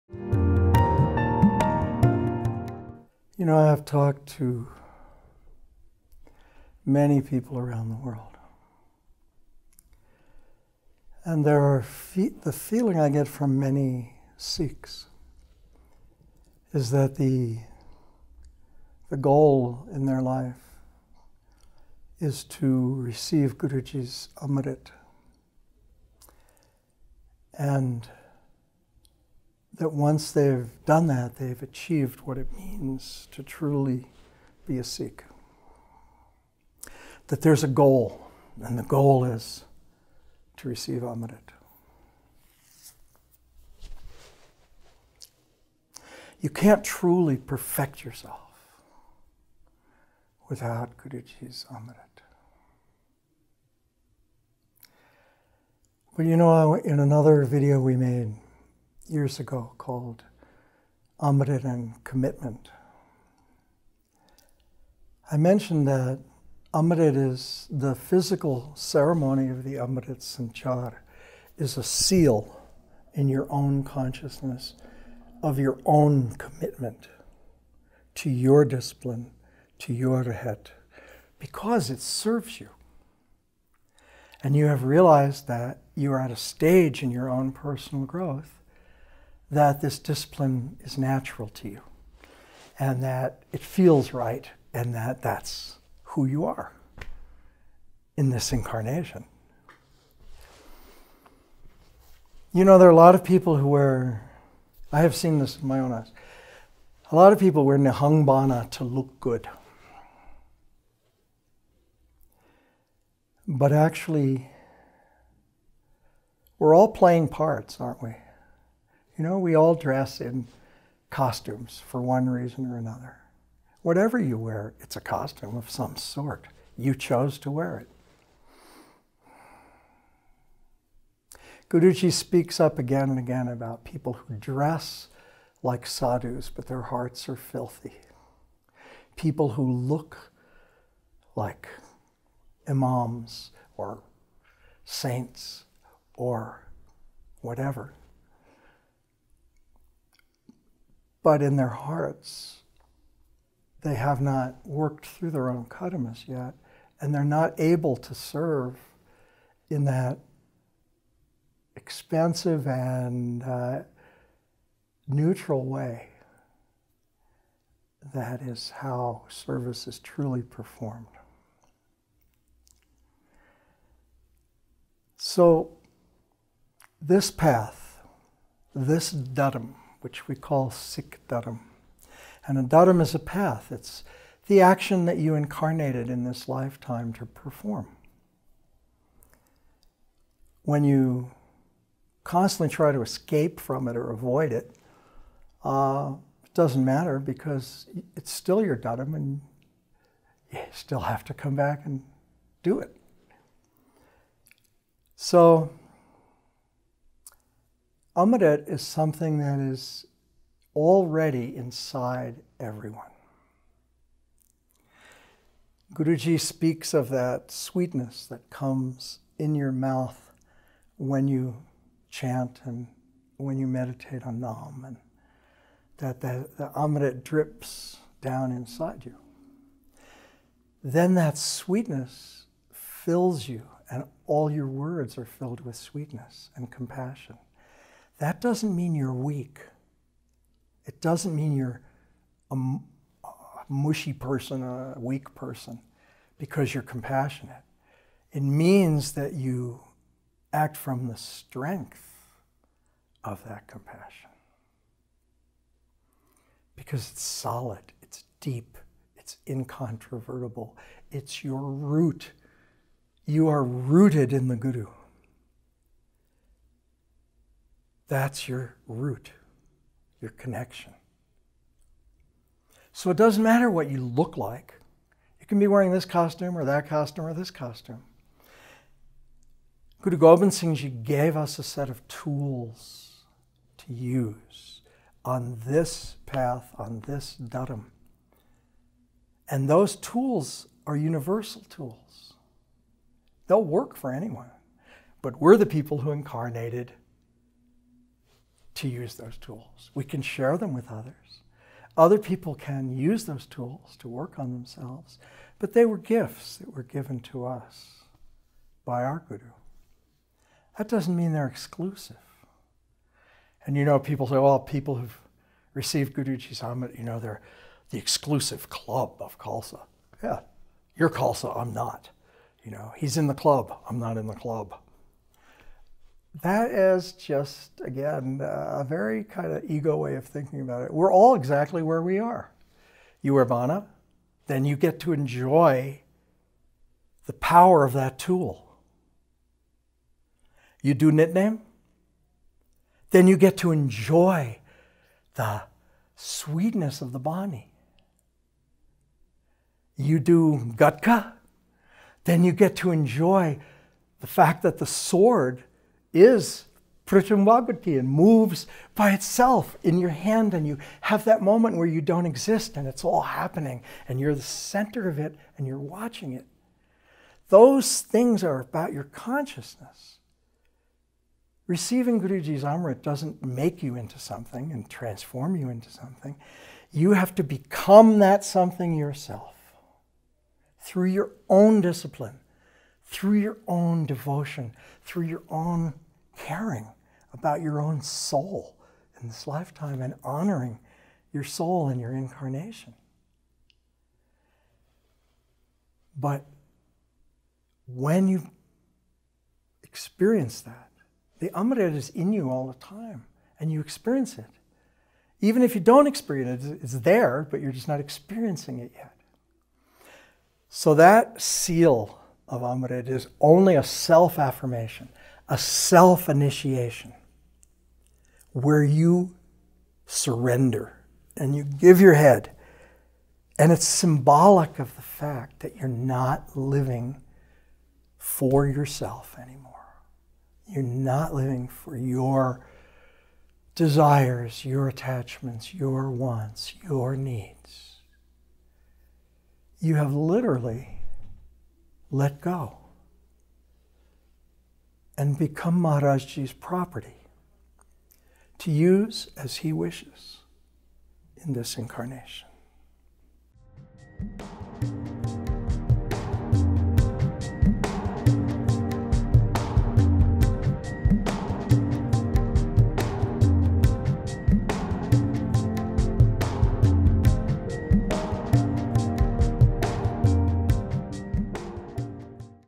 Should Everyone Take Amrit? Guruka Singh Answers
Guruka Singh discusses the topic of "Should Everyone Take Amrit" in this video. As he explains, many Sikhs have a goal in their life is to receive Guruji's Amrit. He added "Receiving Guru Ji's Amrit doesn't automatically make you into something special. You have to become something special yourself."

Like the work, SikhNet is doing? Help us to create more such content.

Download Official SikhNet Mobile Apps